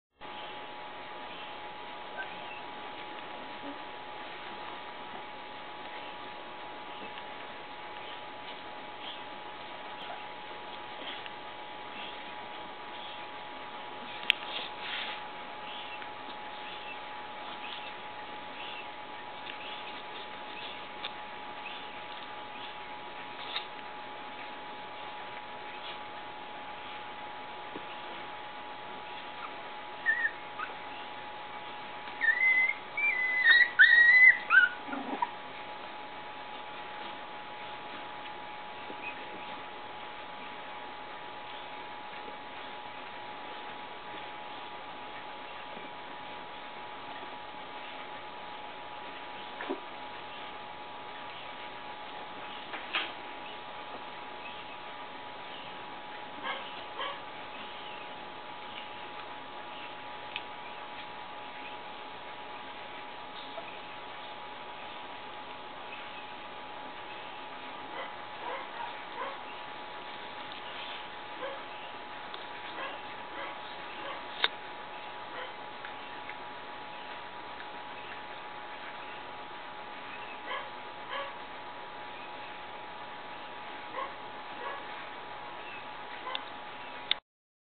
Tessa's 3/4 cavalier puppies at 4 weeks
Fat and sassy, nursed by Basset Penny.